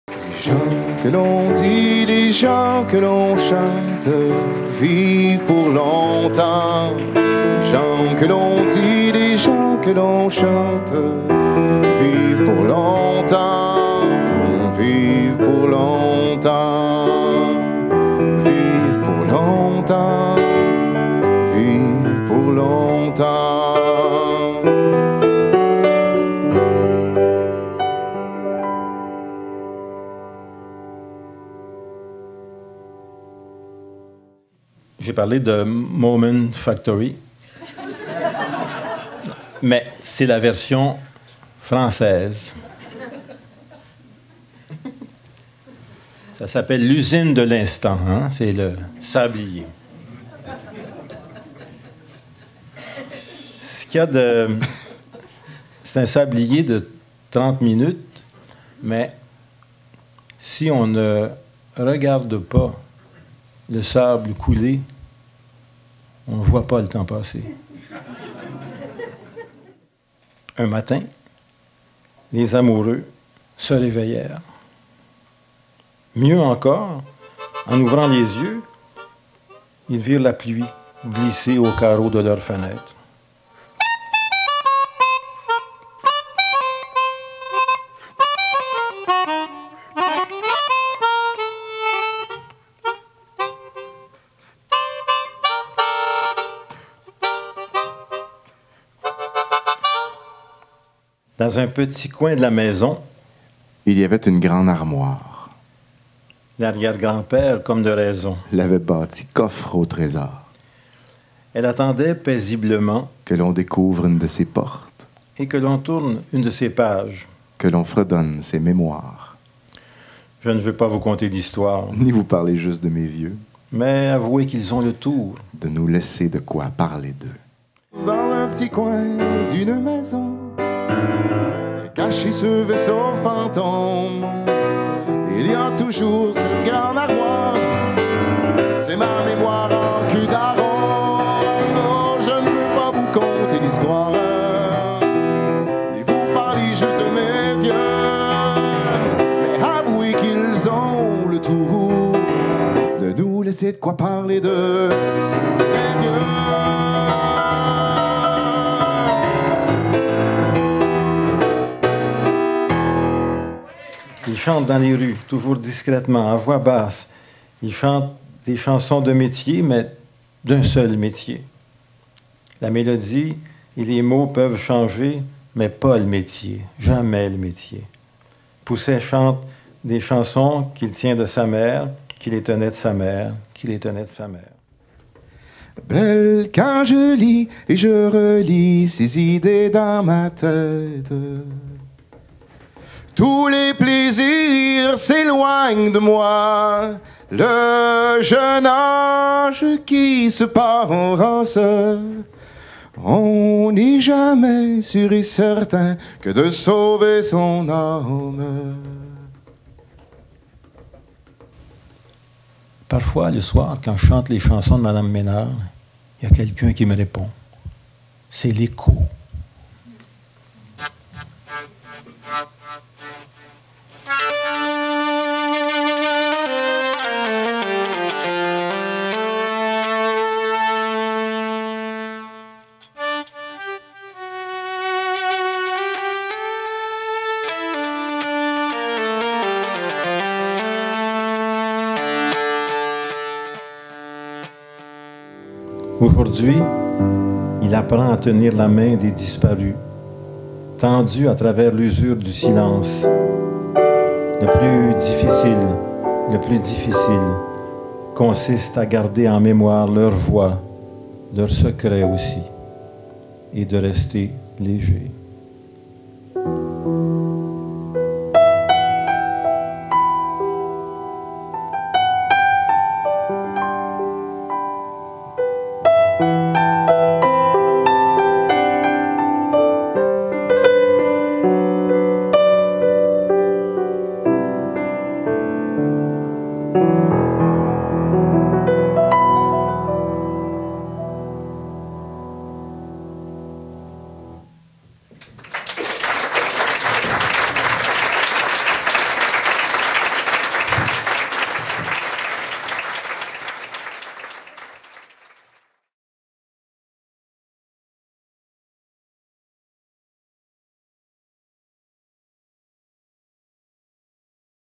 Les Carnets de Notes
Jacques & Nicolas Boulerice
6oct2017 au théâtre Outremont, Montréal
Contactez Geneviève St-Denis
Agente de spectacles et relationniste